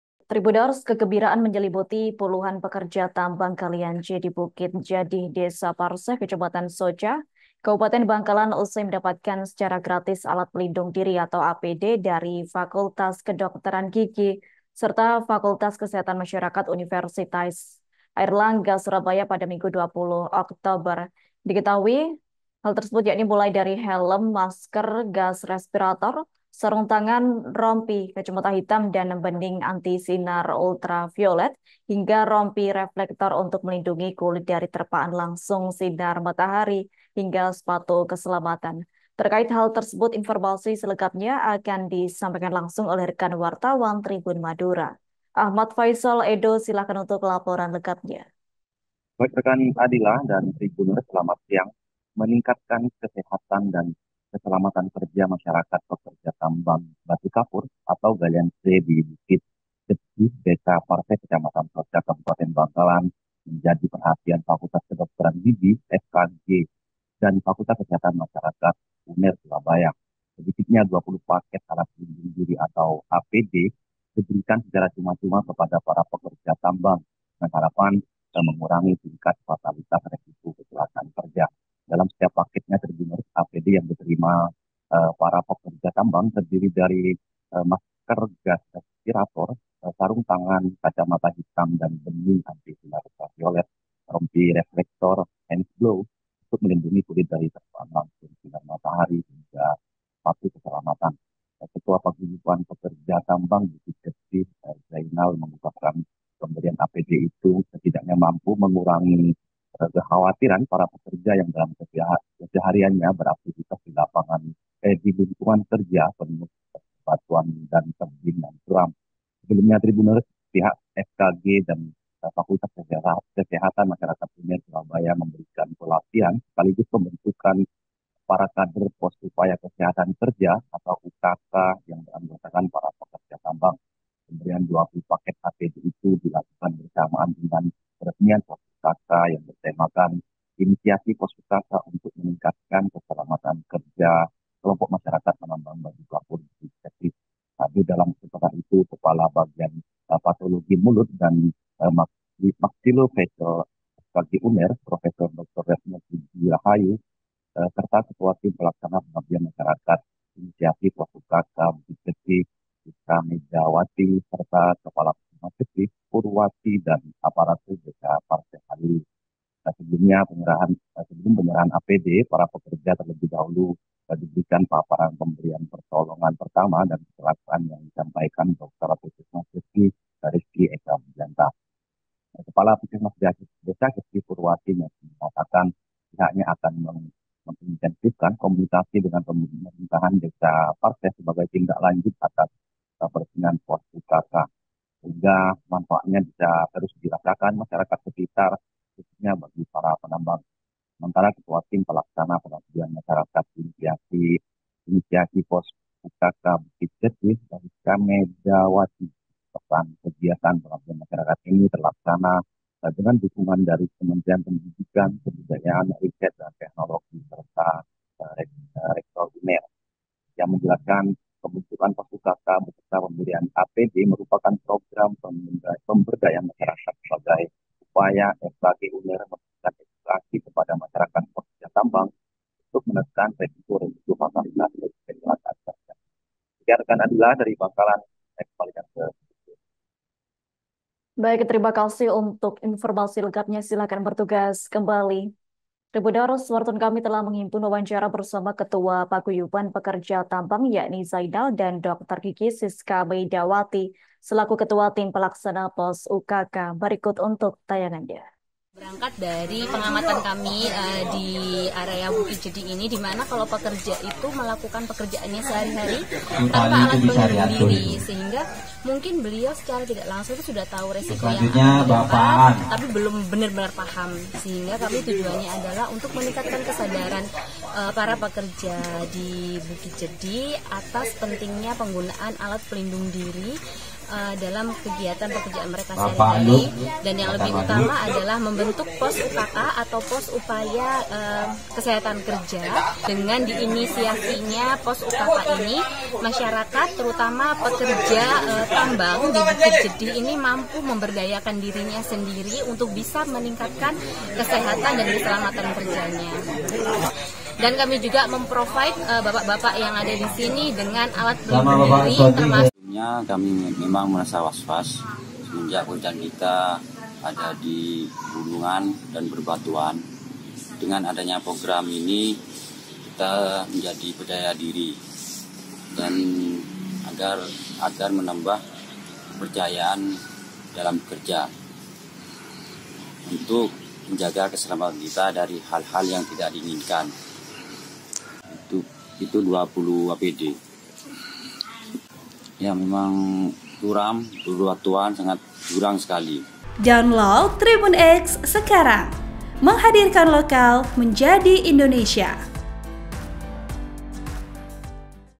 Dapat APD Gratis dari FKG UNAIR, Pekerja Tambang Galian C di Bangkalan Makin Semangat Bekerja
Laporan wartawan, Tribun Madura - Ahmad Faisol Edo
TRIBUN-VIDEO.COM - Kegembiraan menyelimuti puluhan pekerja tambang galian C di Bukit Jaddih, Desa Parseh, Kecamatan Socah, Kabupaten Bangkalan usai mendapatkan secara gratis alat pelindung diri (APD) dari Fakultas Kedokteran Gigi  (FKG) serta Fakultas Kesehatan Masyarakat Universitas Airlangga (Unair) Surabaya, Minggu (20/20/2024).

Mulai dari helm, masker gas respirator, sarung tangan, rompi, kacamata hitam dan bening anti sinar ultraviolet, rompi reflektor, hands blow untuk melindungi kulit dari terpaan langsung sinar matahari, hingga sepatu keselamatan. (*)

Program: Live Update
Host: Adila Ulfa Muna Risna
Editor Video: Ika Vidya Lestari
Uploader: Dyah Ayu Ambarwati